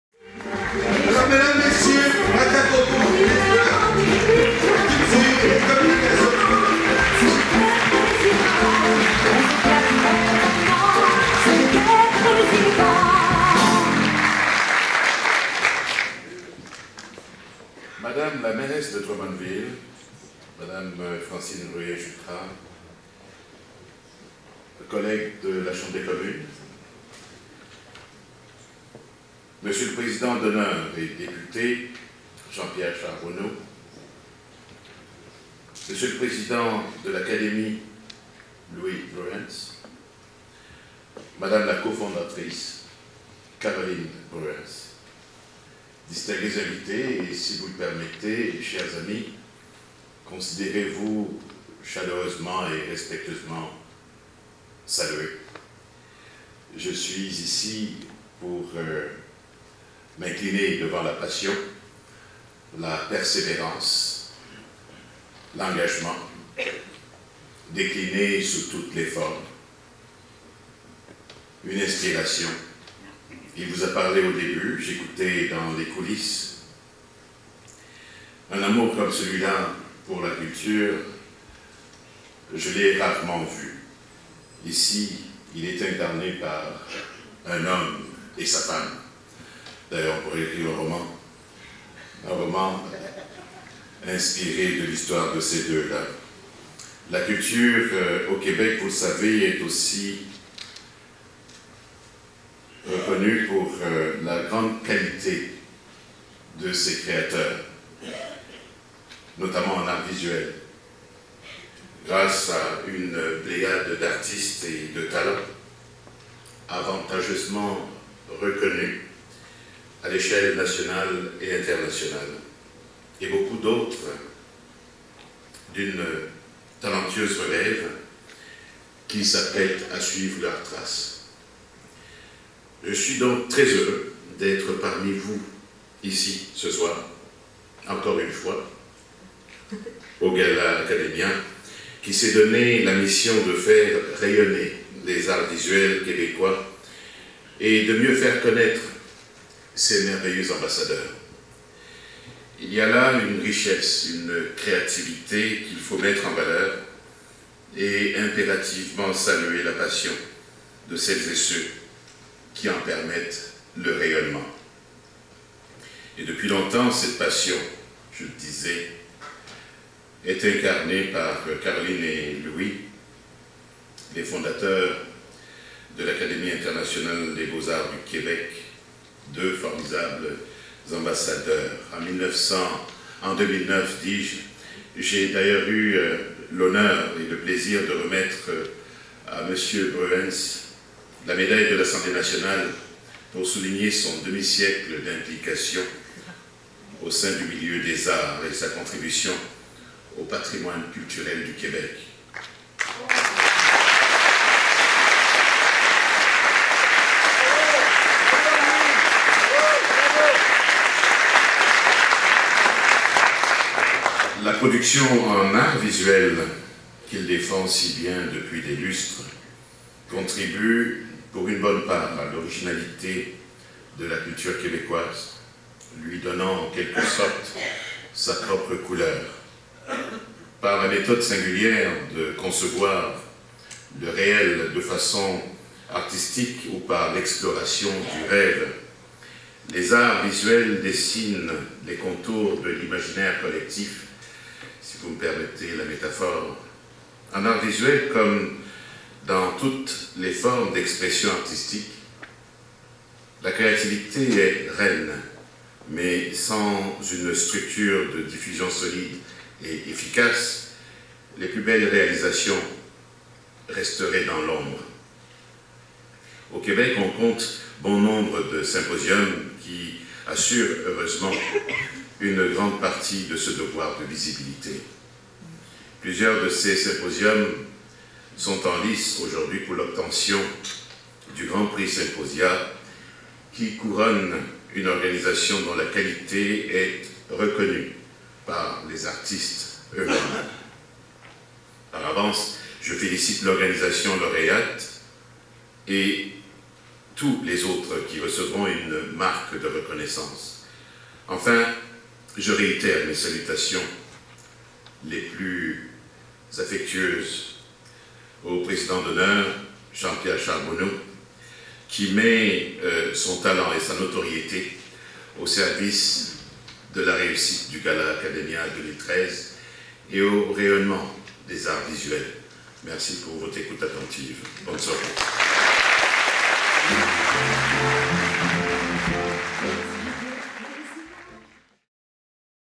AIBAQ - Gala 2013 - Discours de Maka Kotto
Discours de Maka Kotto, Ministre de la culture et des communications du Québec, au gala Académia XXI 2013, de l'Académie internationale des Beaux-Arts du Québec, à Drummondville le 11 mai 2013